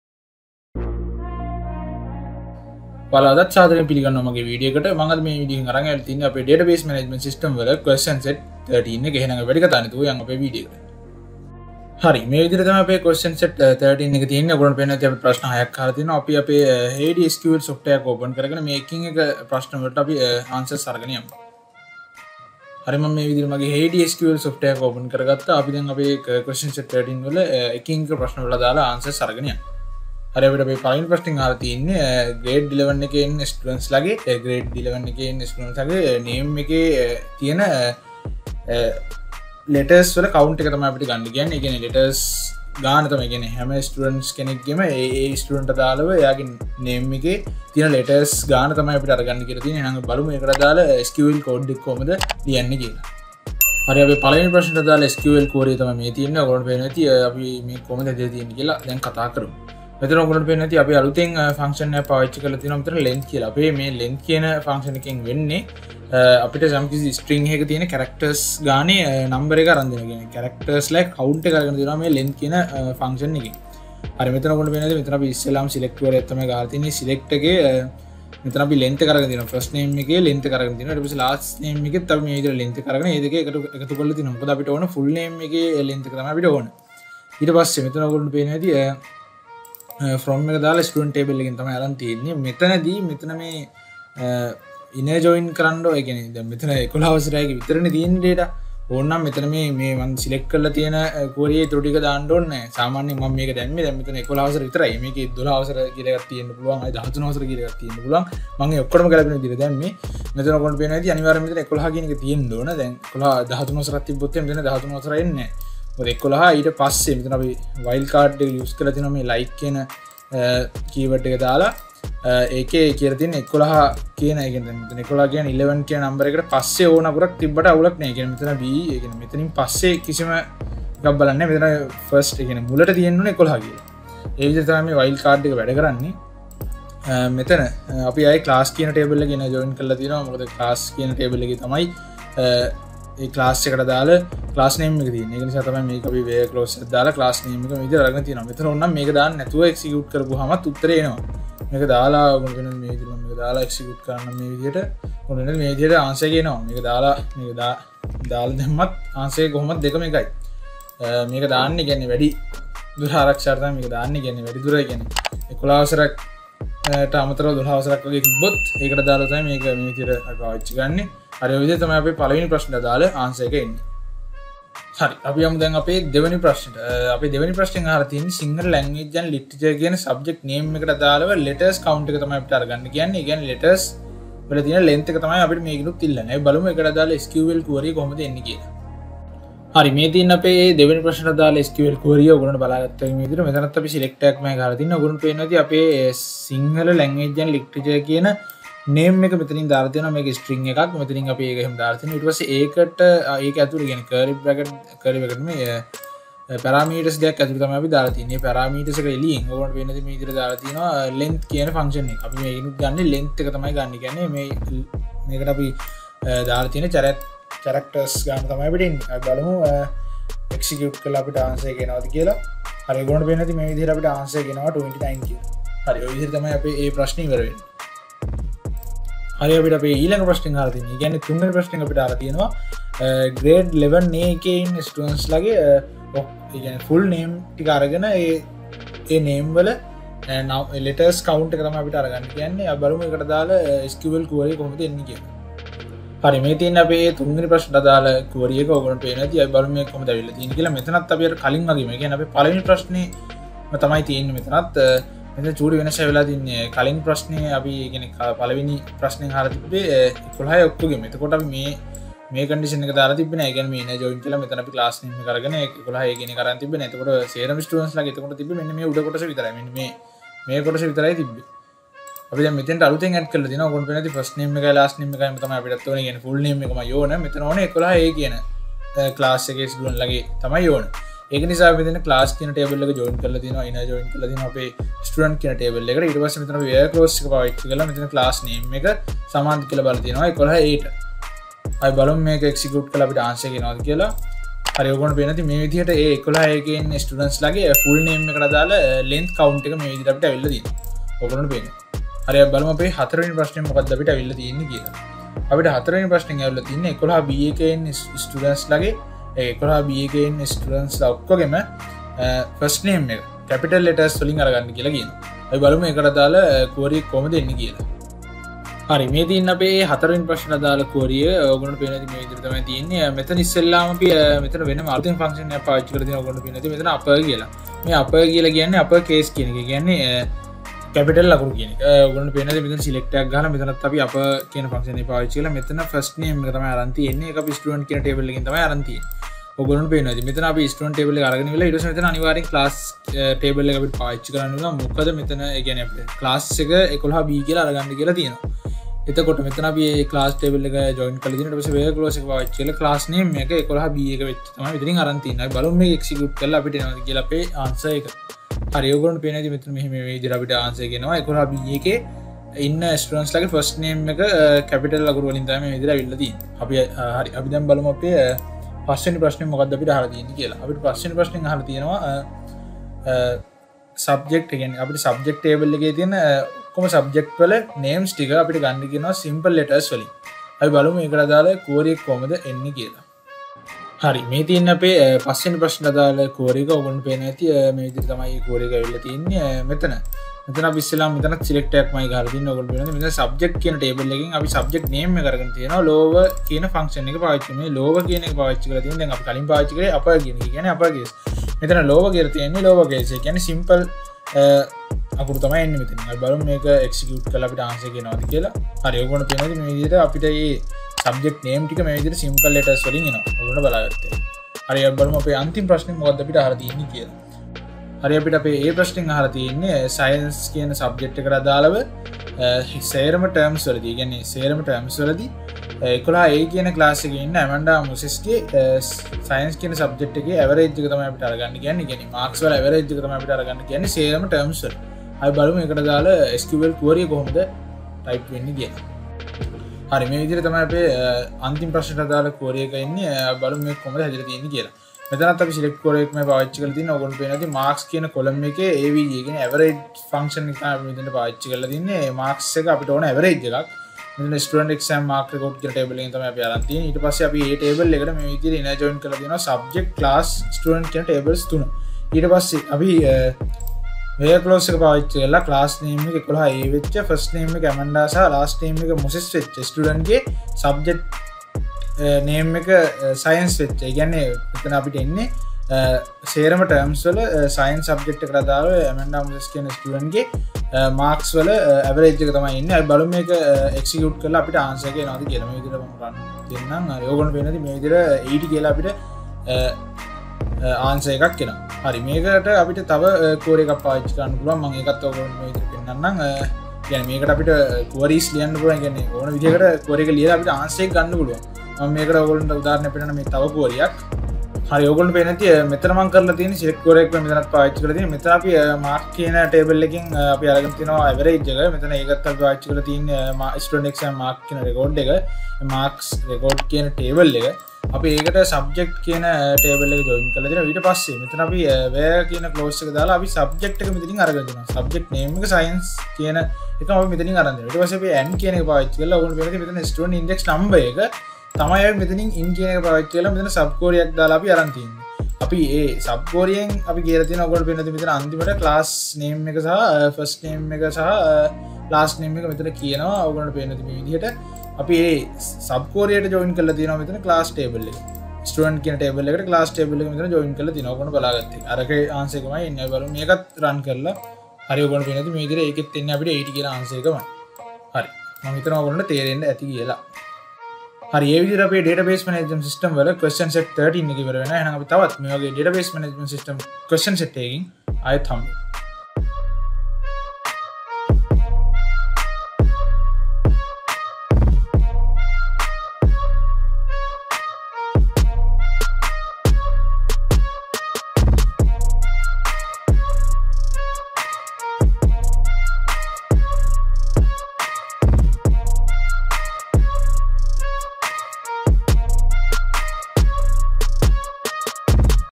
Question Set 30 | Database Management System | Java Institute
This problem describes how to write the query statement used for CHAR_LENGTH,CONCAT, UPPER, LOWER, and AVG SQL functions and the inner join query used to join tables.